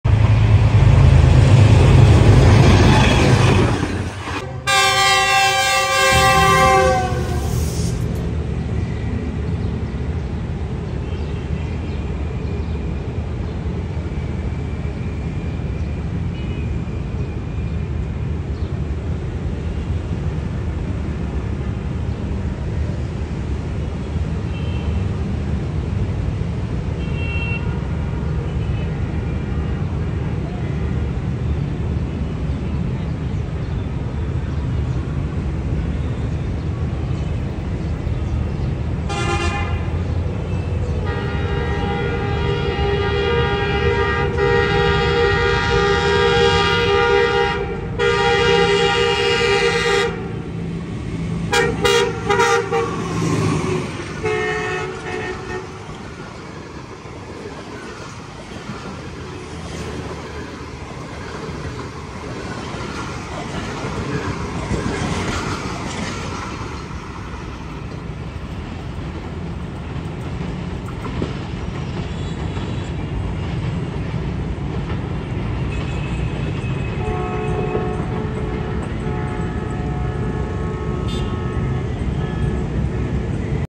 7up Tezgam Express lead by ZCU20(6402)|Pakistan Railways 2nd High Speed Train passing Gujranwala
Pakistan 2nd Luxurious Train between Karachi and Rawalpindi